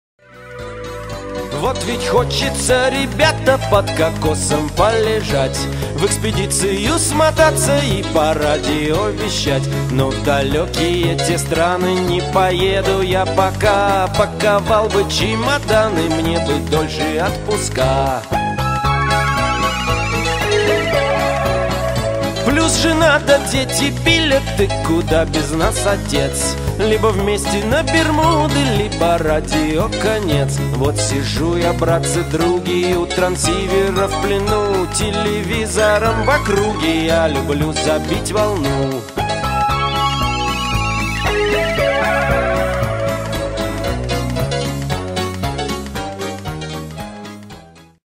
VP2MAC VP2MXT VP2MLJ Остров Монтсеррат. С сайта dxnews.com
AC8W, KB8TXZ, N8LJ будут активны с острова Монтсеррат (IOTA NA-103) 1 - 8 октября 2015 позывными VP2MAC, VP2MXT, VP2MLJ.